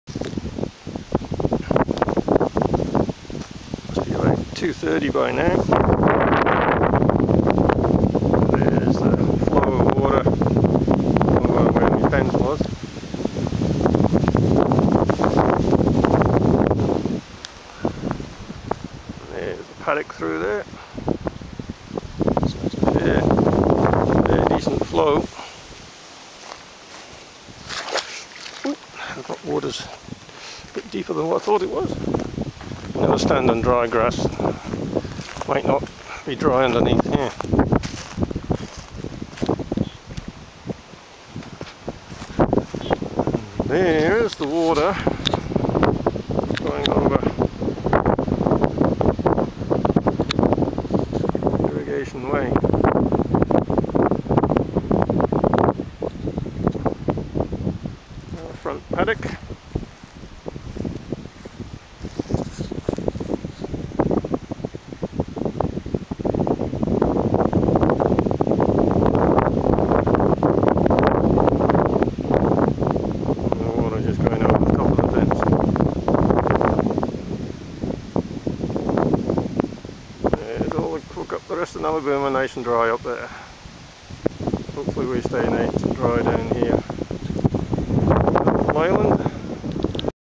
Flood peaked 4.30 pm 9th march 2012
Flood has peaked in town river 10.30 peaked Nallabooma  4.30 pm 650 mm below 1974 flood level at Nallabooma